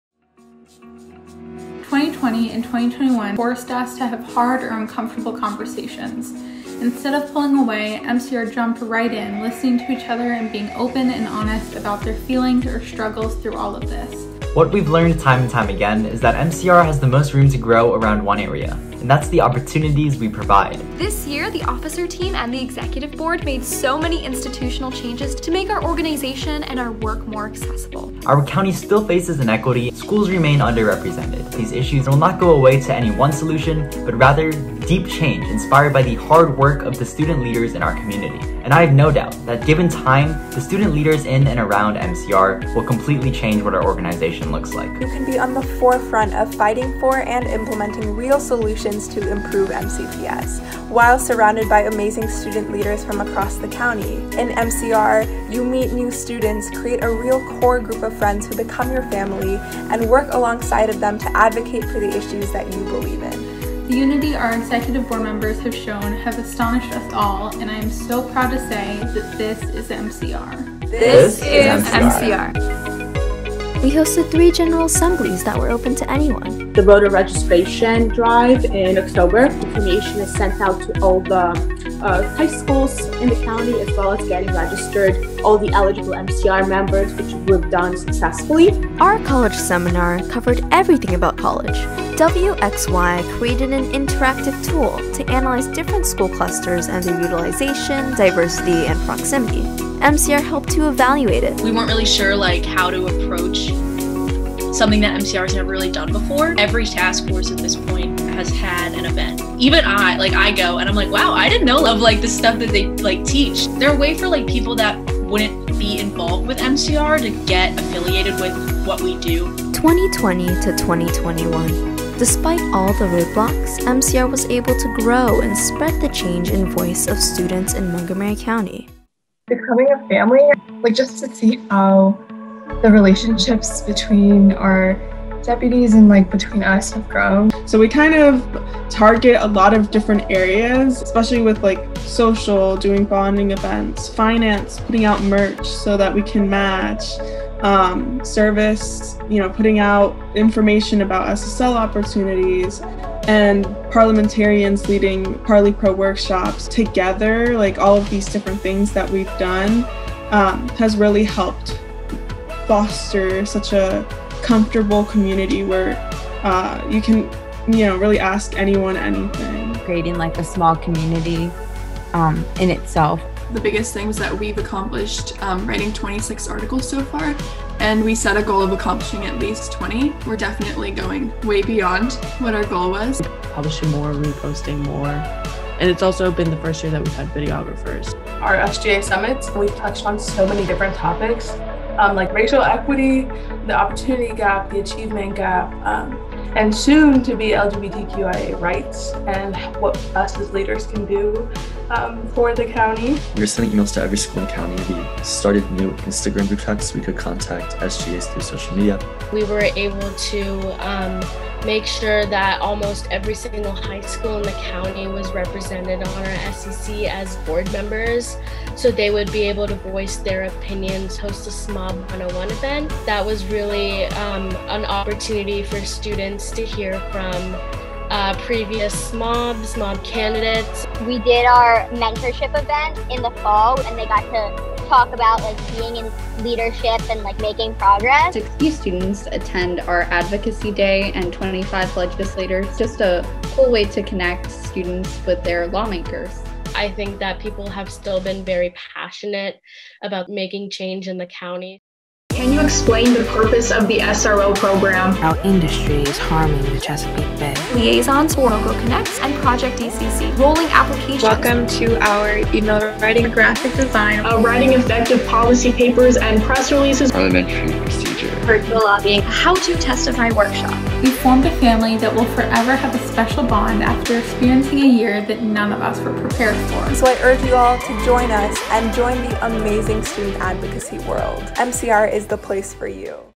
MCR-SGA Year in Review 2020-2021
A look back at the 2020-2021 school year for the Montgomery County Regional Student Government Association (MCR-SGA). A year filled with unexpected challenges, the officer team, executive board, and general assembly adapted, organizing major structural reforms and numerous virtual events.

Video edited by Lizette Miranda and Lindsay Gantman. Featuring members of the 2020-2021 MCR Executive Board.